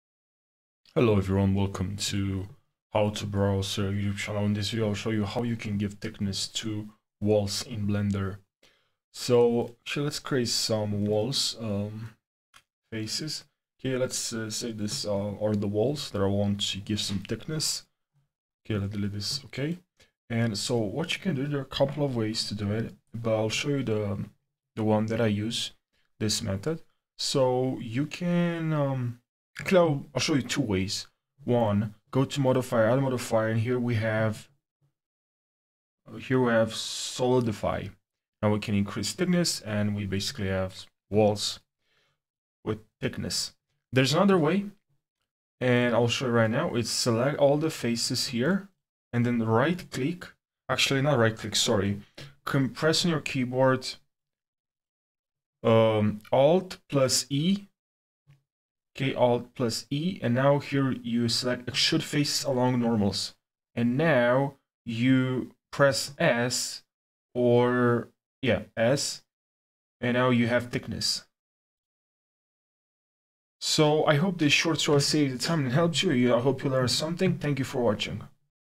Blender Tutorial: How To Add Thickness To Objects In Blender
You won't want to miss a single detail of this tutorial since I will take you step-by-step through the entire procedure. So please follow along with me and watch the entire video on how to add thickness to objects in blender